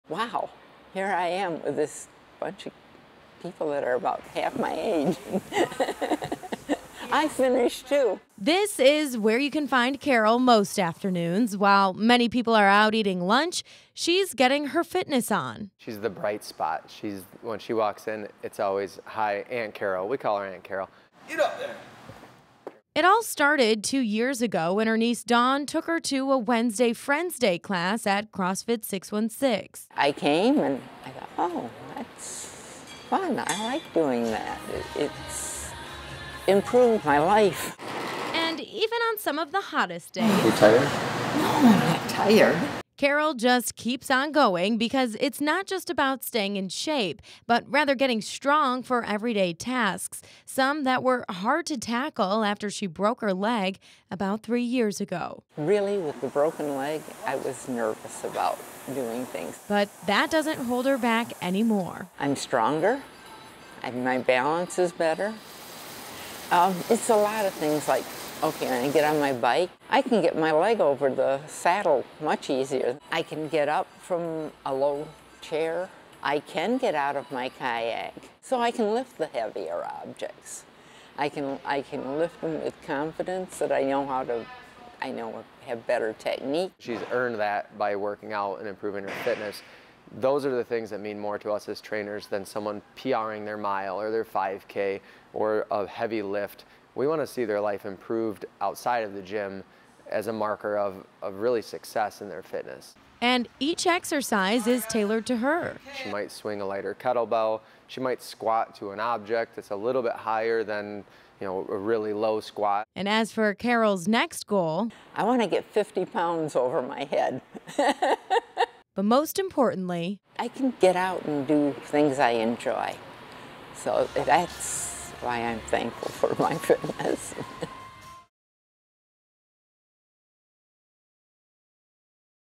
78-year-old CrossFit Athlete
Carol Leigh or "Aunt Carol: who does everything from Yoga to CrossFit while proving age is just a number.